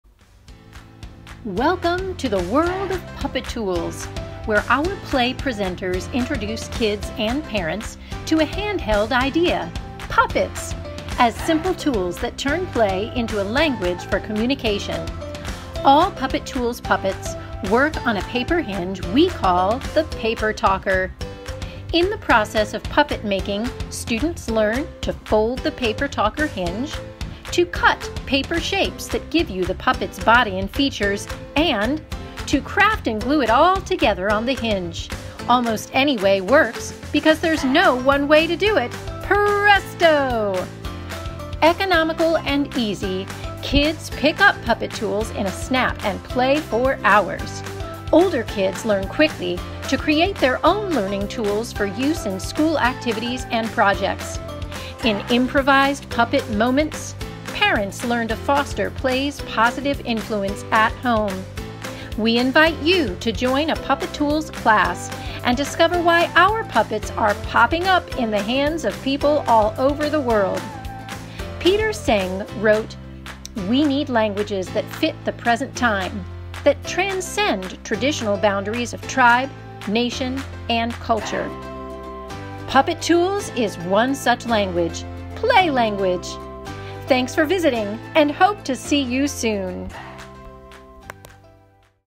Puppetools LIVE Trailer
This video introduces you to Puppetools or 'Play Language,' a LIVE make & take workshop session for adults and children of all ages.  Visit playtectonics.org